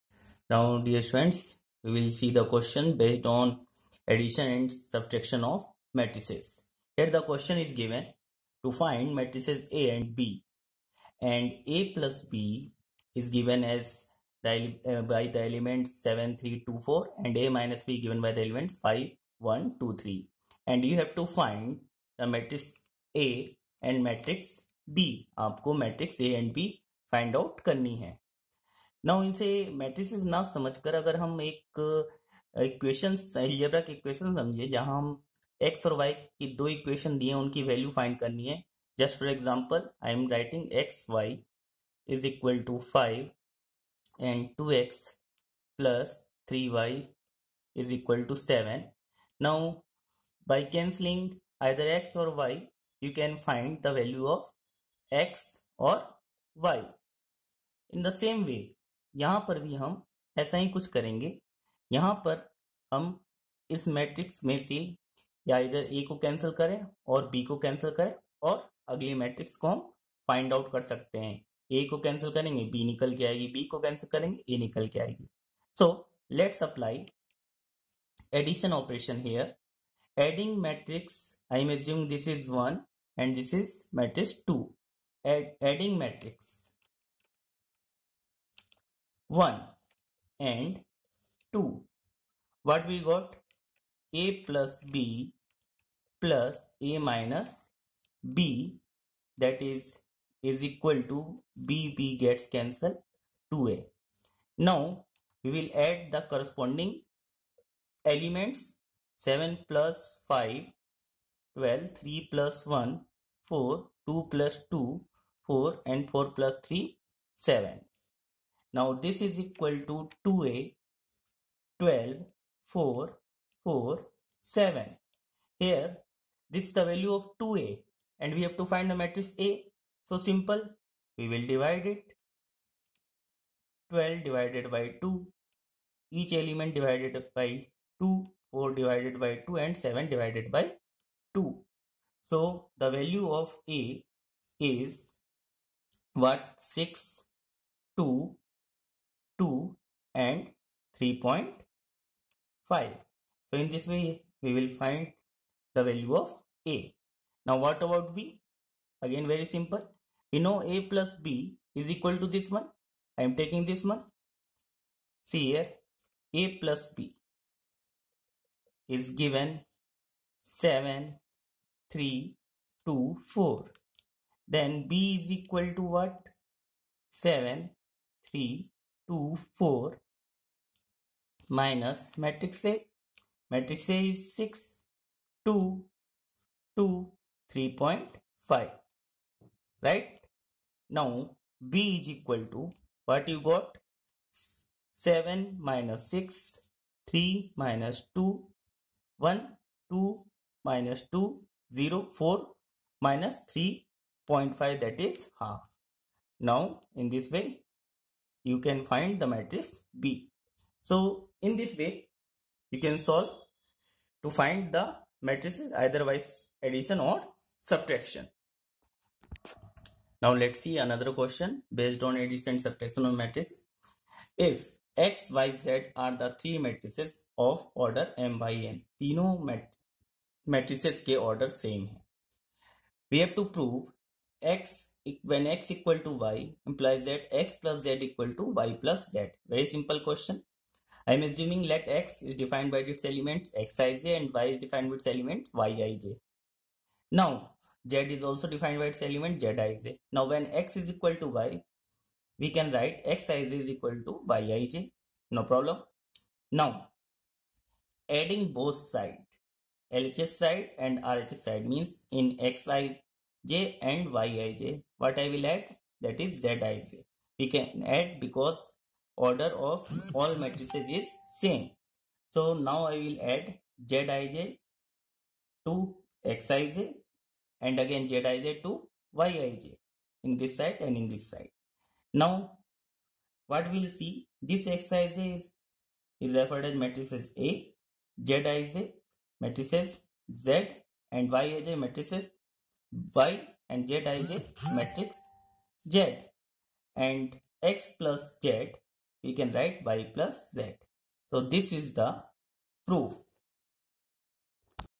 Questions based on addition & Subtraction of Matrices by Physics Funda.
solved question are very helpful for students for self study.

Physics Funda
PHYSICSFUNDA